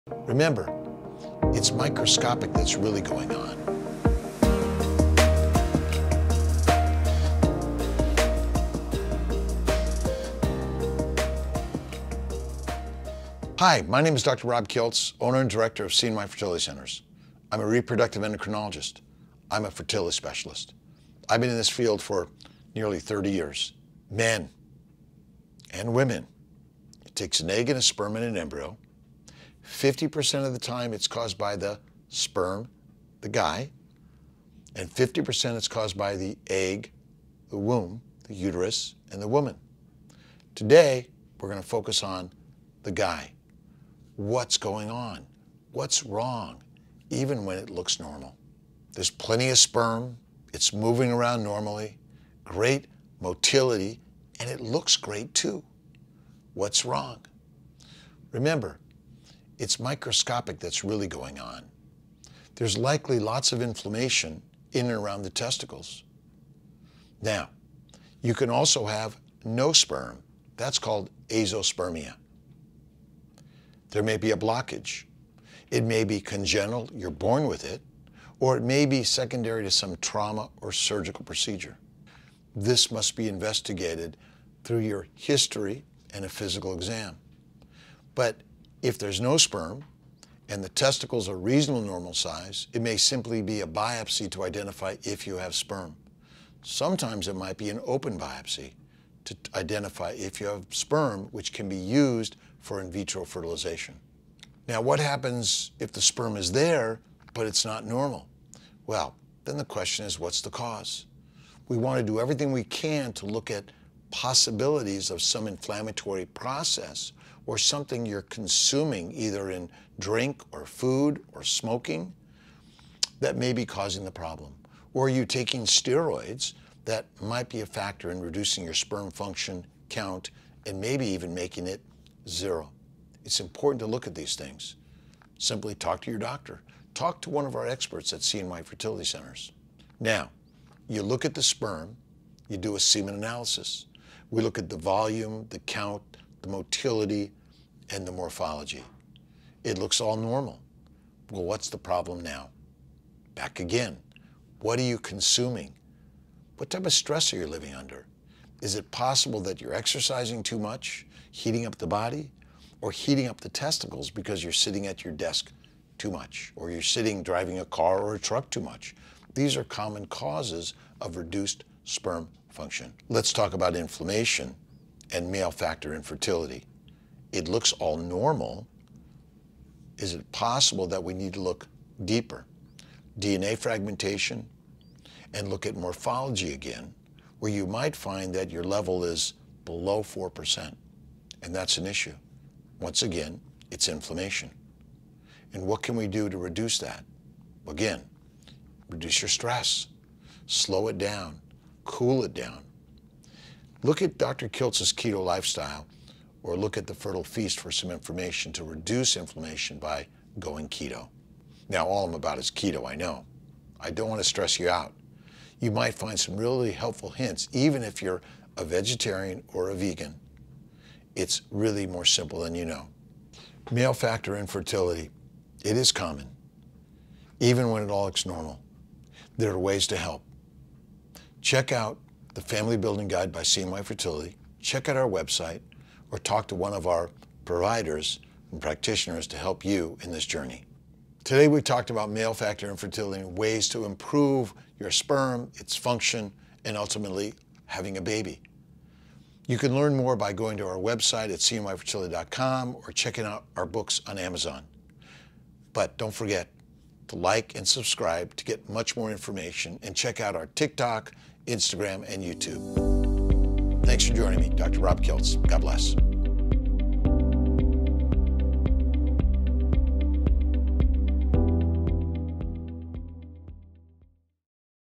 CNY Fertility | How to Improve your Sperm FOR MALES
Although it makes sense, it may still surprise you that half of all infertility cases can be attributed in part or in full to the man (aka male factor infertility). The good news is that most men are constantly producing new sperm and with the right lifestyle changes, a man can significantly increase his sperm count and motility, improve their morphology, and increase their overall male fertility.

For More information on assessing, improving and treating male infertility check out our blog post